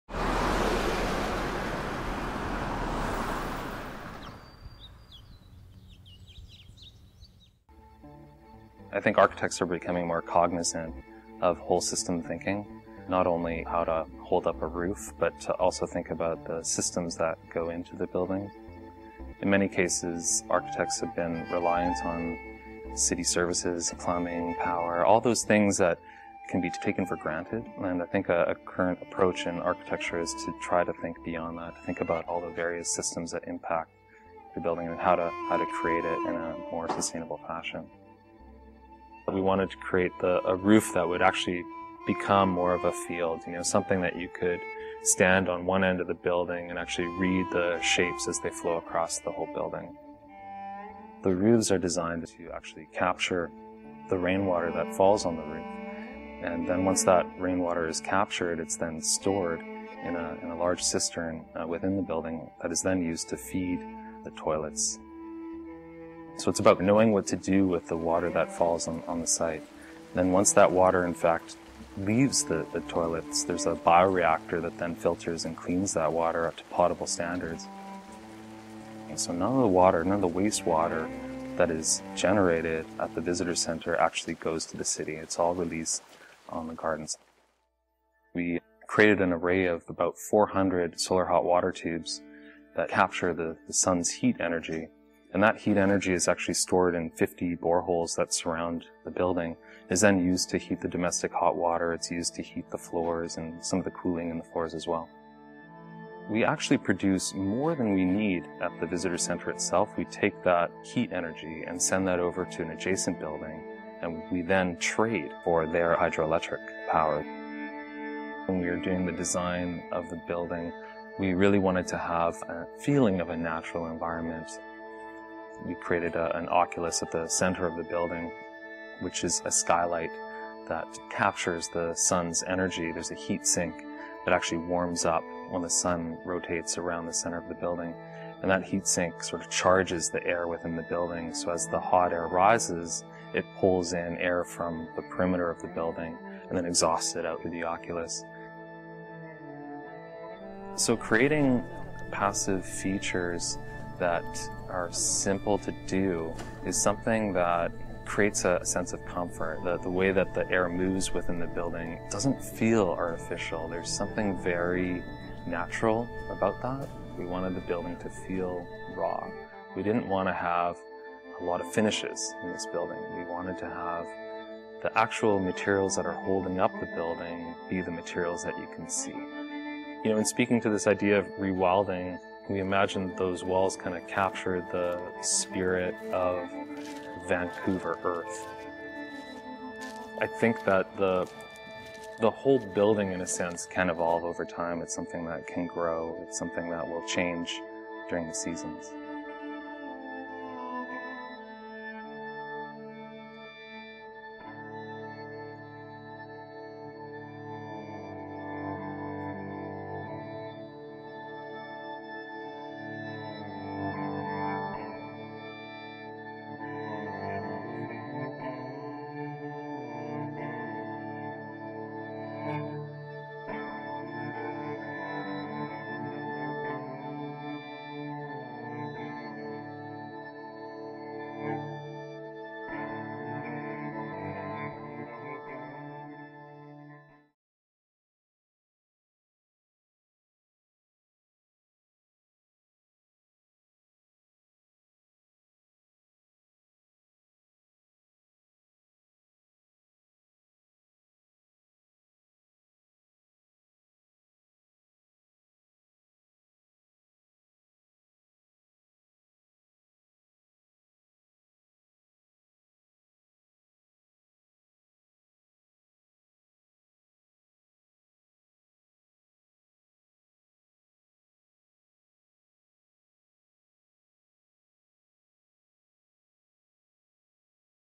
Rewilding Vancouver: VanDusen Visitor Centre
Find out why this has become a landmark green building leading the way to Vancouver's architectural future.

Student project by: Ellen Russell